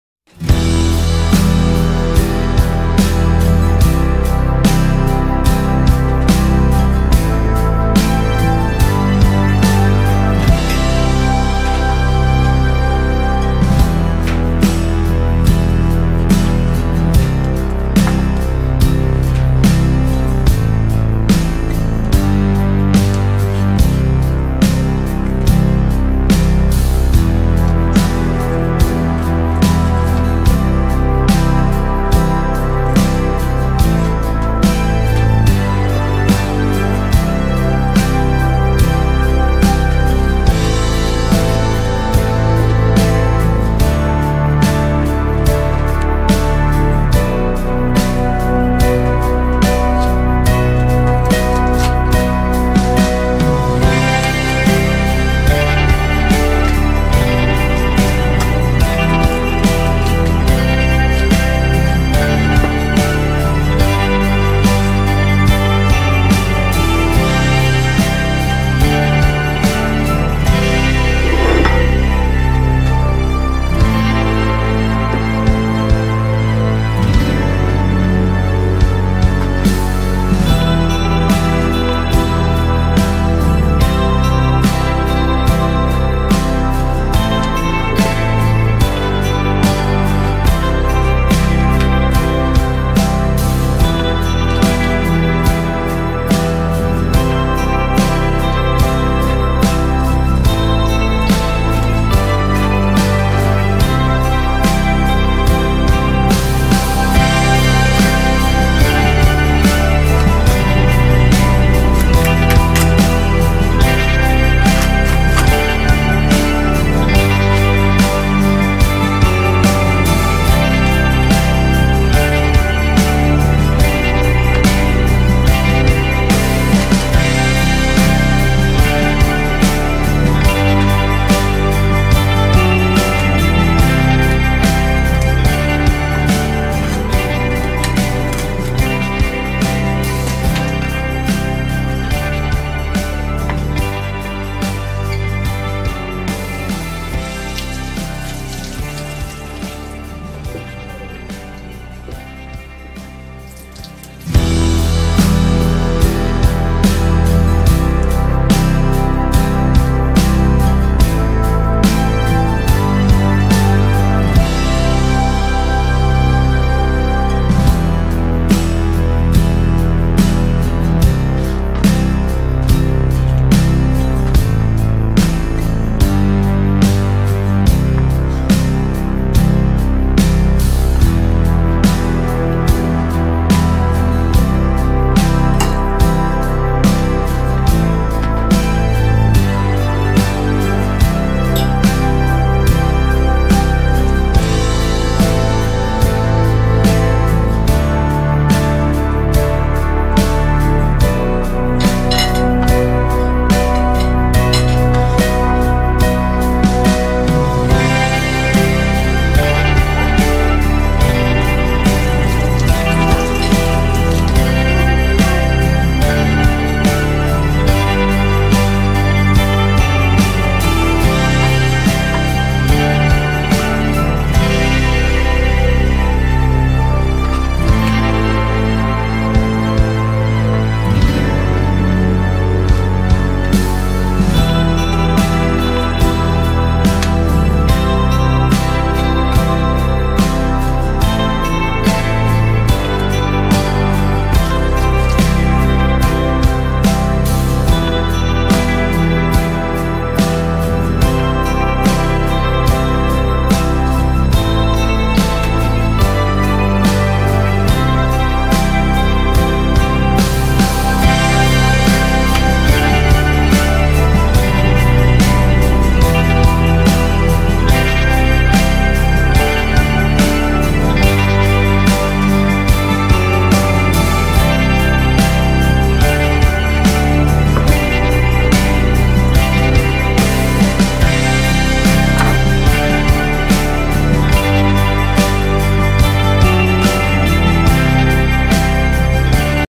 How to fillet a John Dory. Fish filleting. Разделка рыбы. Мастер-класс по разделке Джон Дори.
Hello! Today I want to show how to fillet a John Dory fish! Tips from the professional filleter.
Привет всем! Сегодня я хочу показать вам мастер-класс по разделке рыбы Джон Дори. Смотрите, как правильно разделать рыбу!